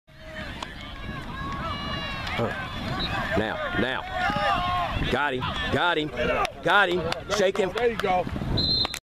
#24 kobe 2 #3 Josh Catch 1 Shake & Bake
GAME 1 VS SAN DIEGO ALL-STARS 2018 AYF NATIONAL CHAMPIONSHIPS TOURNAMENT